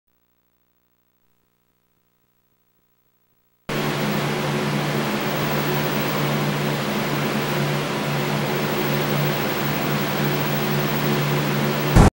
RDMVTL 8024 ~ 8/29/2005 [1997 CCJ MEMORY] STAIRCASE & FRIED BOLOGNA
JUNCTION STUDIO 118 ~ A clip that reminds me of the microwave under the CCJ staircase where everyone fried their bologna .
(G)